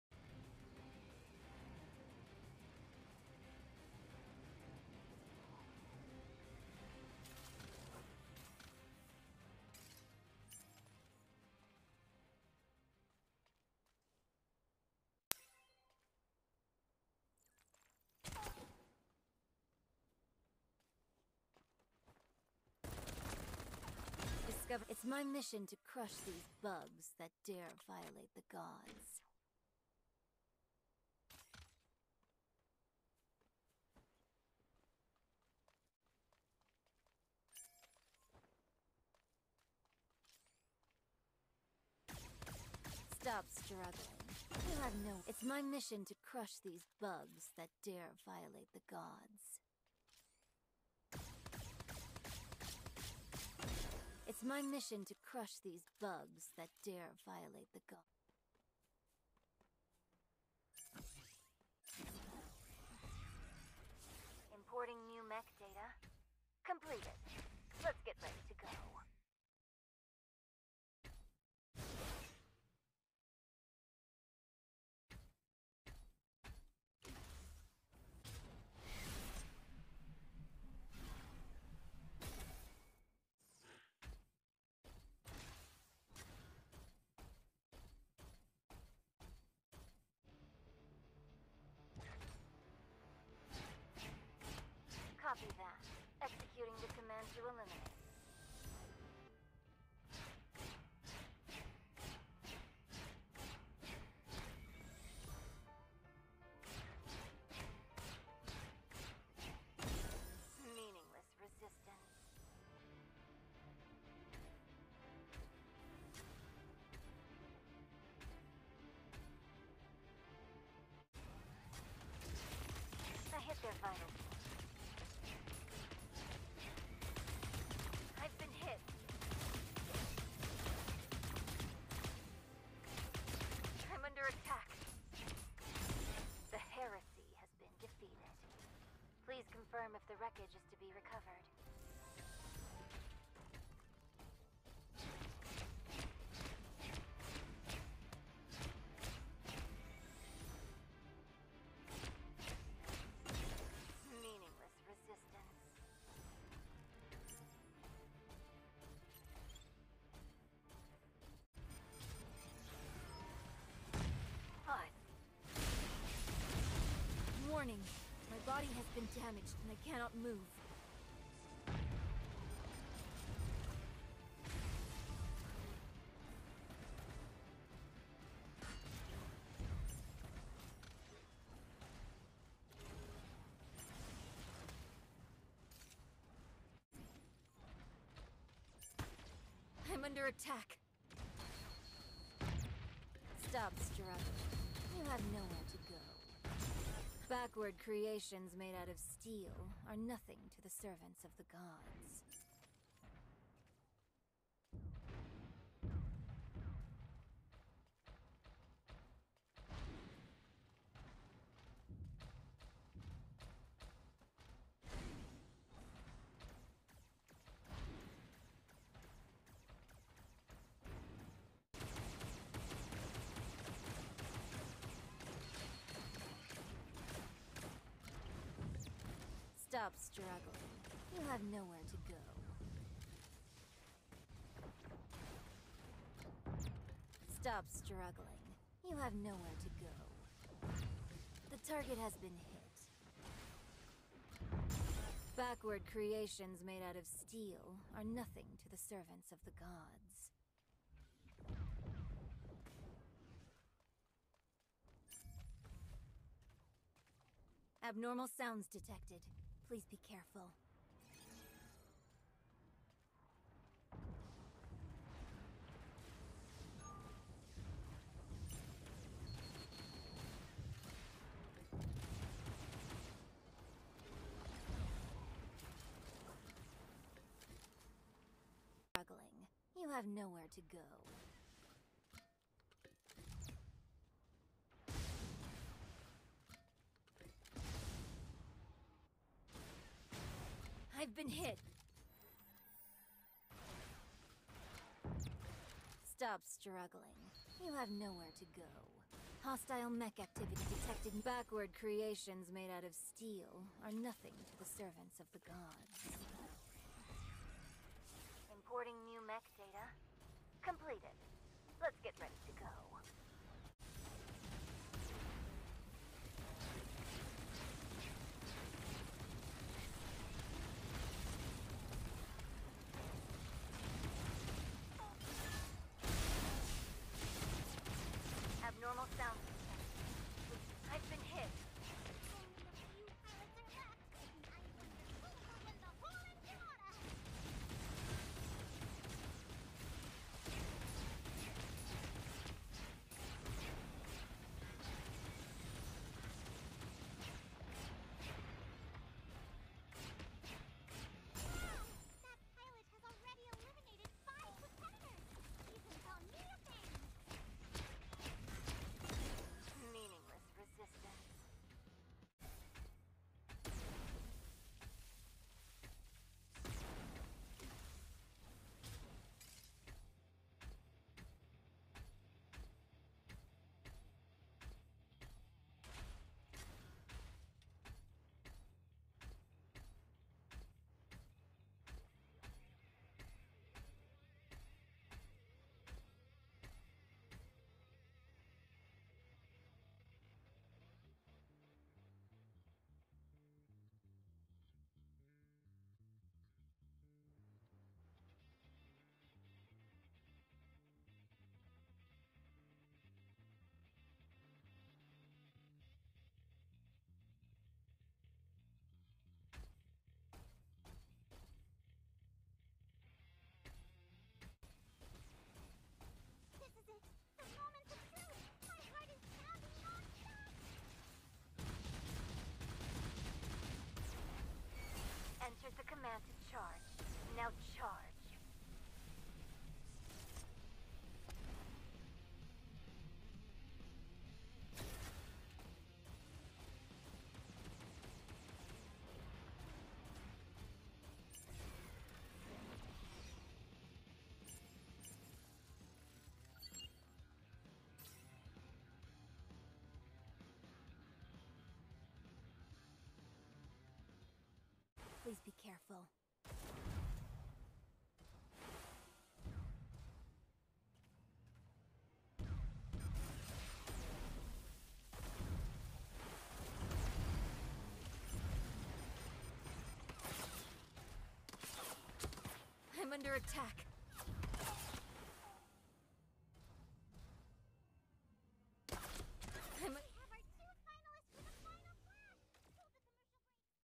First Victim of Death Knell, Scariest Mecha So Far
Finally the new Mecha is coming. It surely has terrible look, just like a mantis. Not yet found the best touch to control the Death Knell but no need to rush just enjoy this.